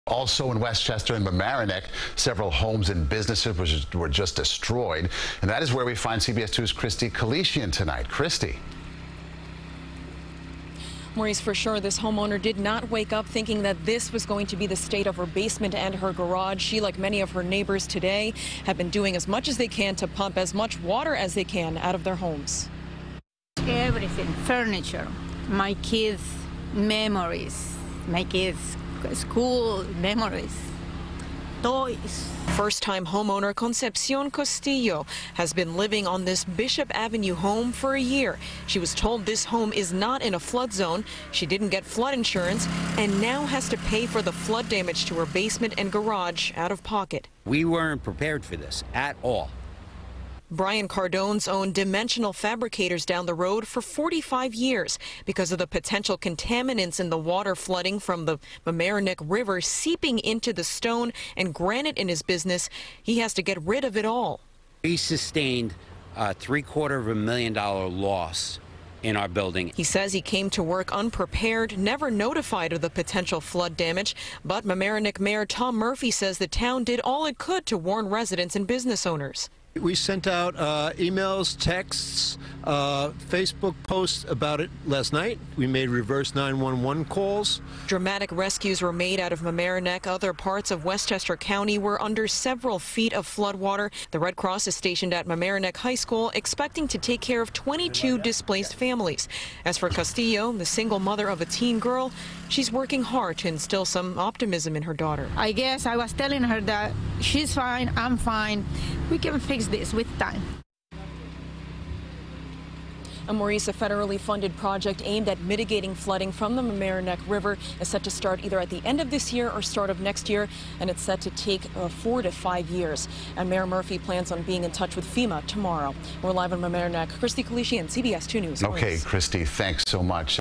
Several homes and businesses destroyed by flooding in Mamaroneck
CBS New York's Kristie Keleshian spoke to residents who are trying to make the best of a very bad situation.